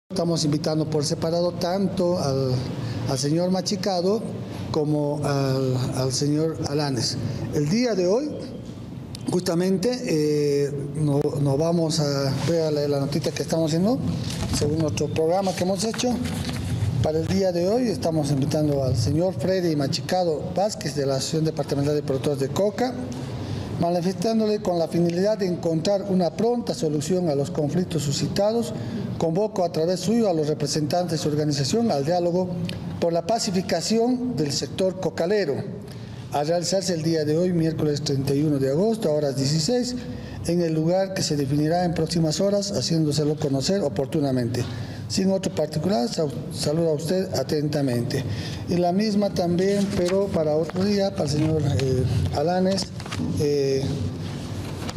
GOBIERNO CONVOCA POR SEPARADO A MACHICADO Y ALANES PARA NUEVO DIÁLOGO.
LaPaz | Gobierno convoca por separado a Machicado y Alanes para nuevo diálogo. Invitan a Machicado para las 16:00 de hoy y agendarán otro día para el sector de Alanes.